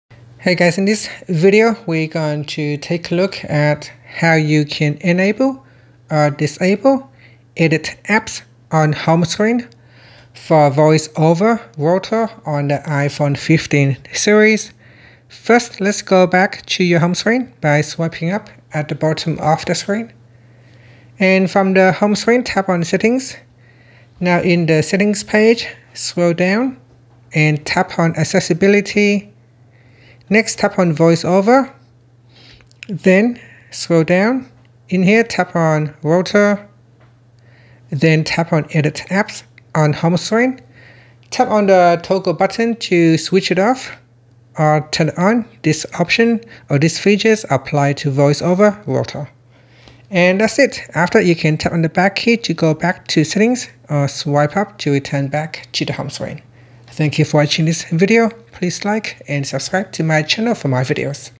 iPhone 15/15 Pro Max: How to Enable/Disable Edit Apps On Home Screen For VoiceOver Rotor
In this comprehensive tutorial, we delve into the accessibility features of the iPhone 15 and iPhone 15 Pro Max, focusing on VoiceOver Rotor settings. Discover how to customize your home screen by enabling or disabling the "Edit Apps" option using VoiceOver Rotor. Whether you're new to accessibility features or a seasoned user, this guide will empower you to navigate your iPhone with greater efficiency.